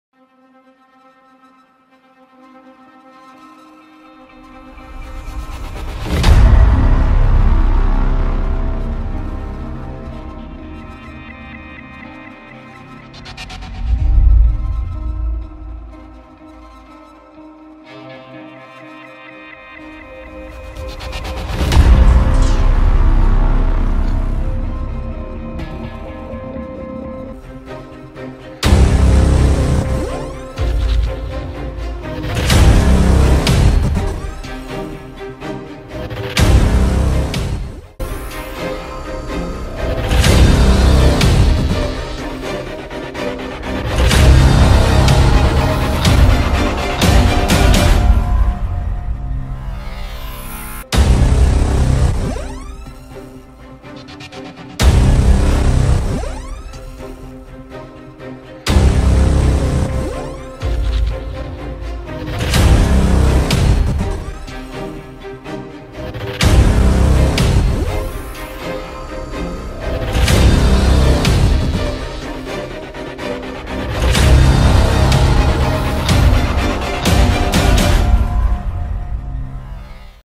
TRAILER DECERTAGE FRANCE PC by Galior
Voici le trailer du decertage, un montage qui consiste à montrer les plus belles actions de notre cher communauté PC !
N'hésitez pas à envoyer vos clips à DRAGONS GooD, qui se fera un plaisir de les prendre (clips en scrim uniquement et pas en league)

Bon visionnage à tous et n'hésitez pas à lacher un pouce bleu !

B3ss0u